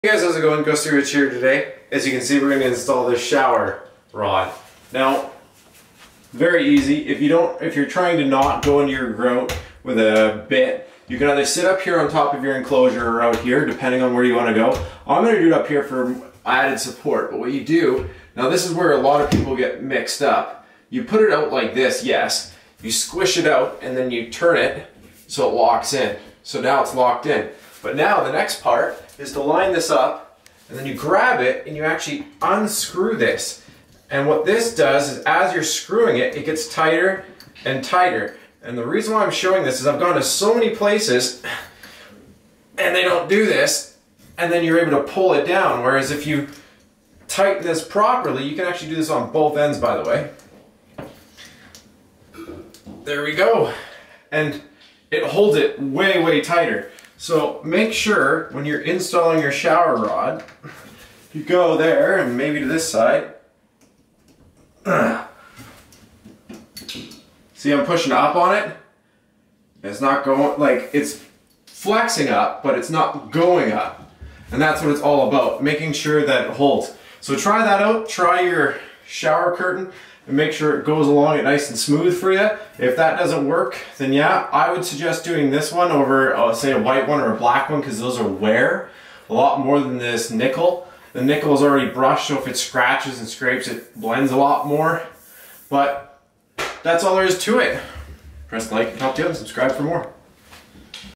HOW TO EASILY INSTALL A SHOWER CURTAIN BAR
This is a quick and easy tutorial on how to install a shower curtain rod or bar. This rod and bar do not require screws and are very cost effective. Hope this diy do it yourself walk through helps you out